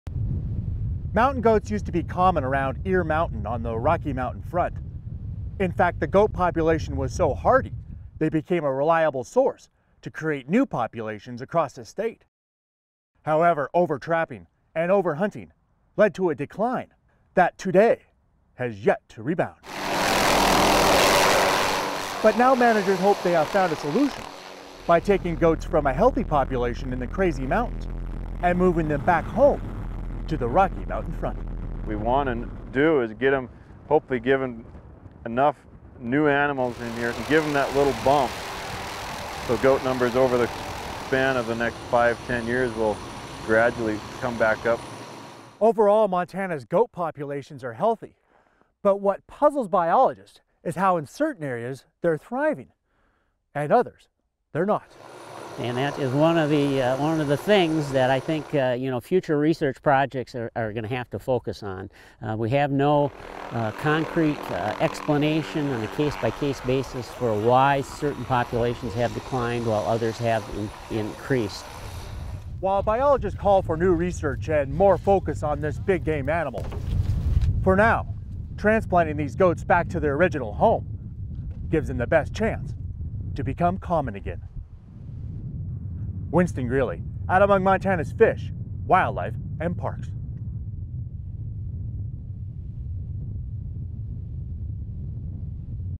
Goat Transfer
Montana Rocky Mountain goats return to Ear Mountain.